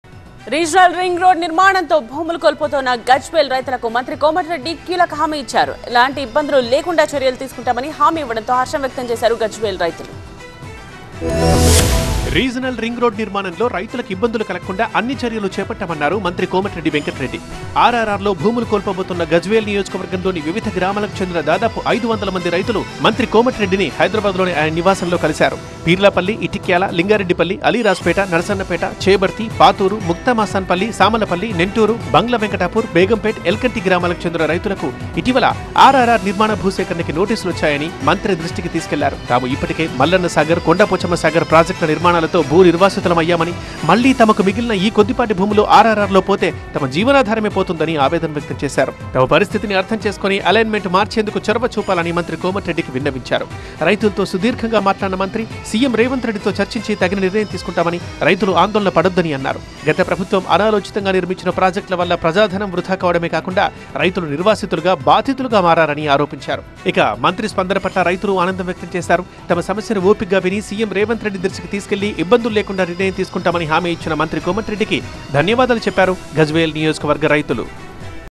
మంత్రి కోమటిరెడ్డి వెంకట్‌రెడ్డిని కలిసిన గజ్వేల్‌ రైతులు - TV9
credits : LOHITH